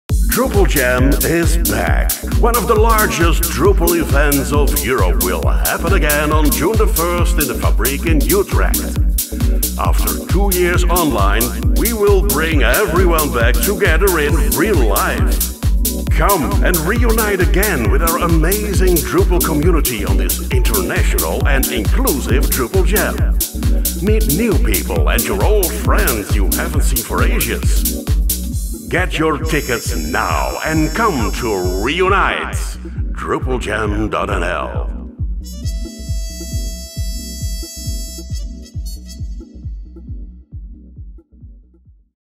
DrupalJam 2022 re:United
The last In Real Life (irl in short) Drupaljam event was in 2019 with over 500 enthusiastic visitors. Then COVID came and we had to quickly adapt to an online event. While Drupaljam re:Boot and the joint event with DrupalCon Europe were a great succes, everyone is longing to get back together.

We will once again push boundaries to bring you an exciting event packed with traditional community talks, keynotes and of course the legendary Drupaljam PubQuiz.

Make sure you clear your agenda for June 1st and join us again in our familiar venue in Utrecht.